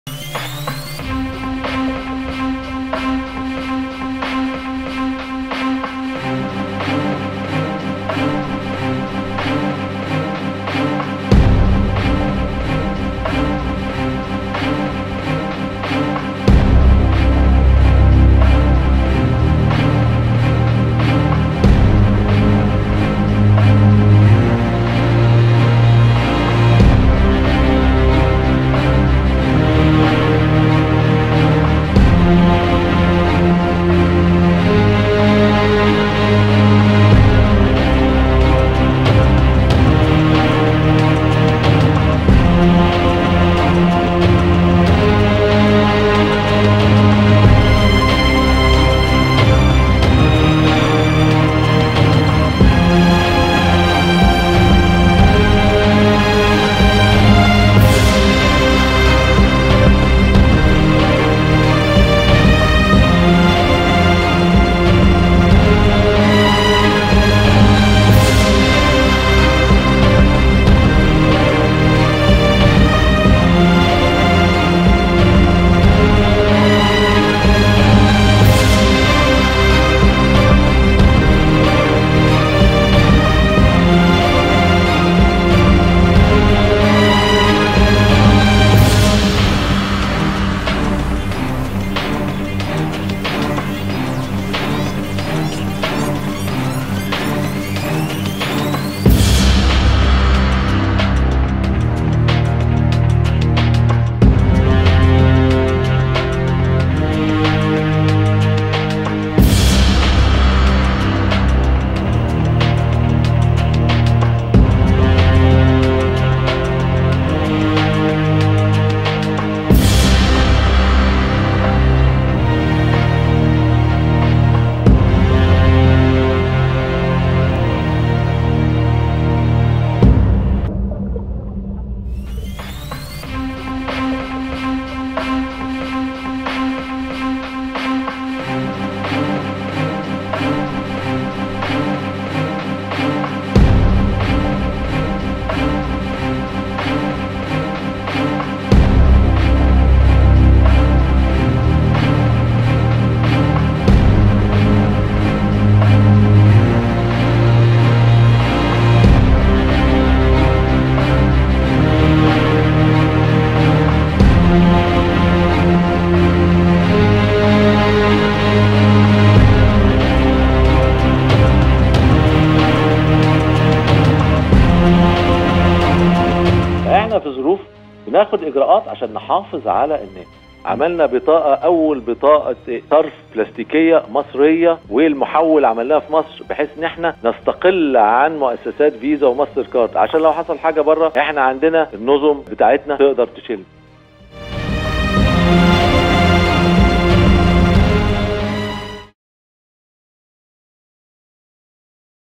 كيف واجه الجهاز المصرفي فيروس كورونا؟ | جريدة المال
حزمة من القرارات الحاسمة أصدرها البنك المركزي في أعقاب انتشار فيروس كورونا المستجد «كوفيد-19»، كان من أبرزها، إلغاء رسوم السحب عبر ماكينات الصراف الآلي، وتأجيل أقساط القروض لمدة 6 أشهر.
نستعرض في هذا التقرير أبرز القرارات التي اتخذت لتحفيز ودعم الاقتصاد والصناعة في مواجهة وباء كورونا.